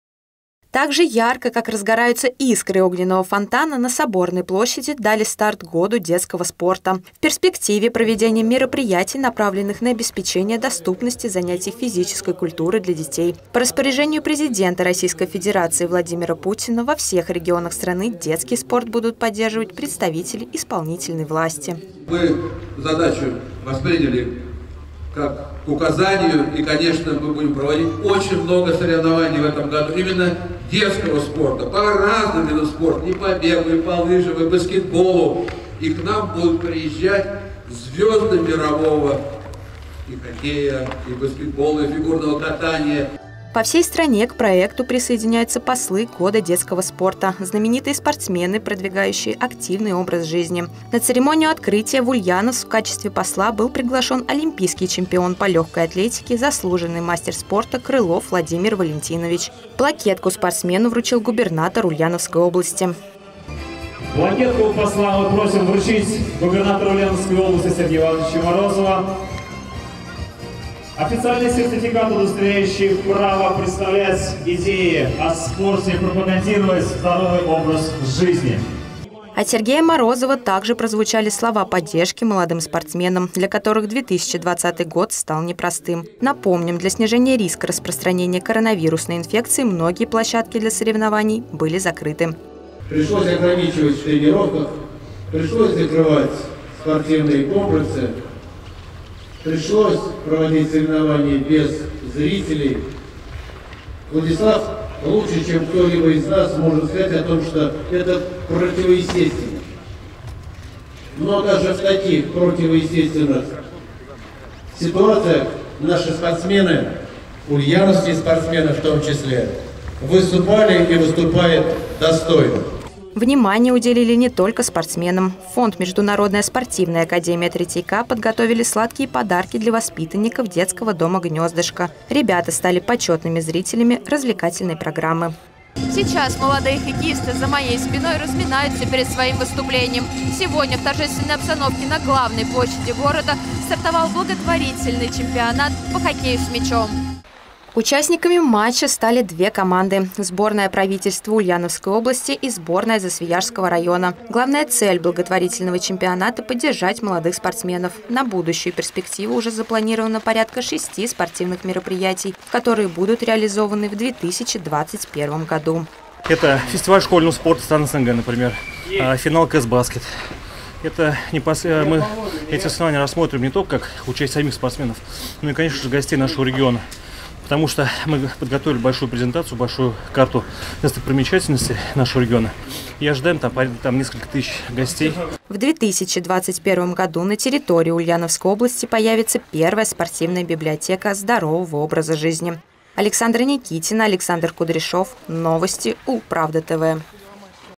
Для всех и каждого. Год детского спорта объявляется открытым
На Соборной площади дали старт Году детского спорта. В перспективе – сделать доступными занятия физической культуры для всех ребят.